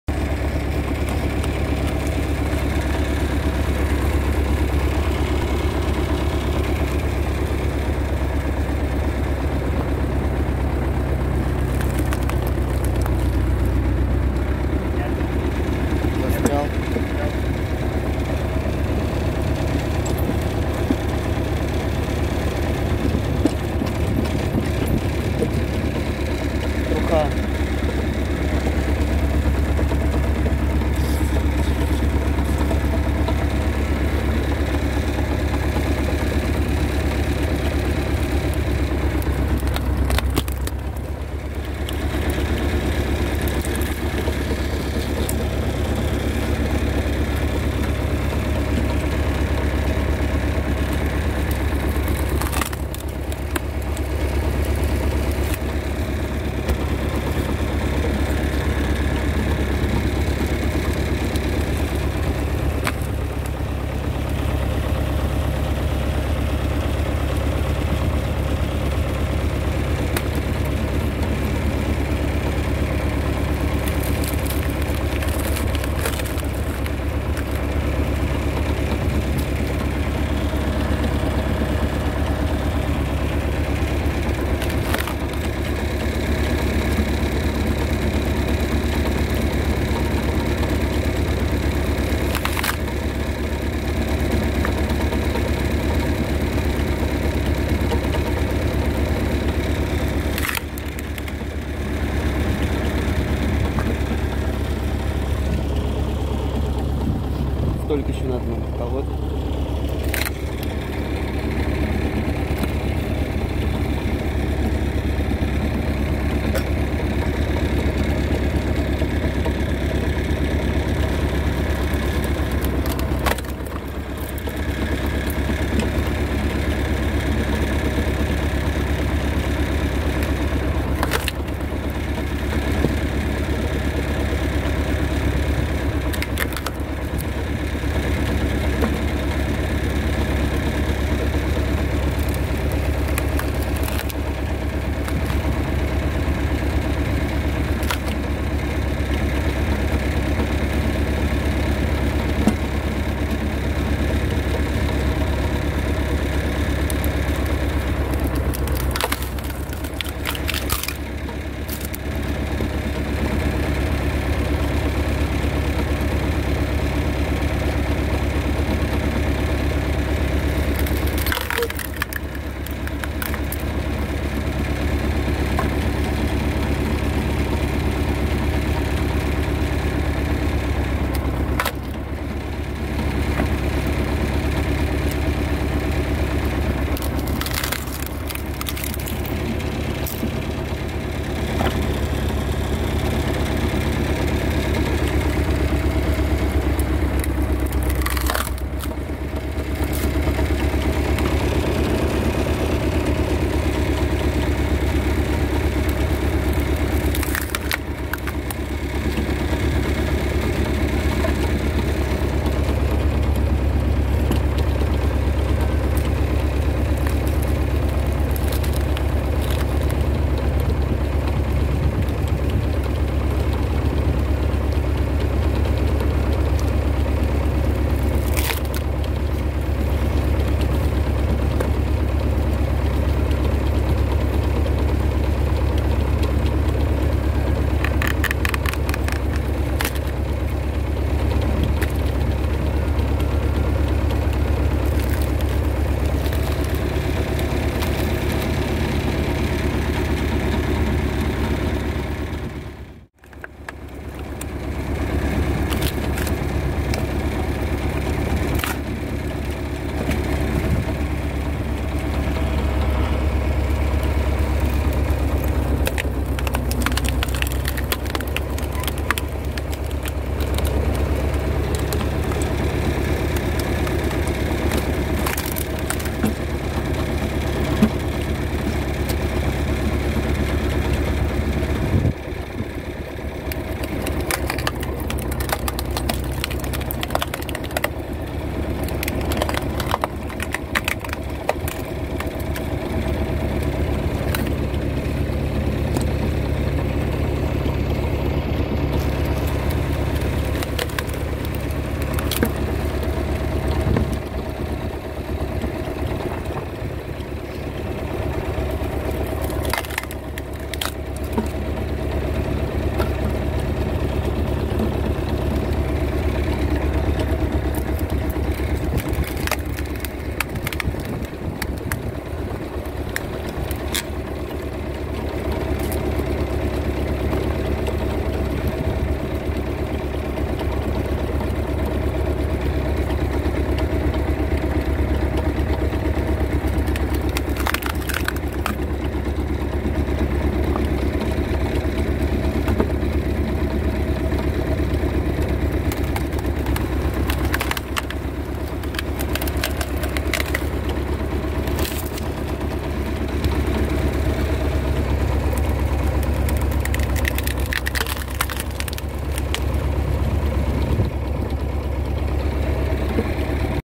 Самодельный дровокол на трактор. Легко колем большие поленья. Как быстро колоть дрова
как сделать дровокол своими руками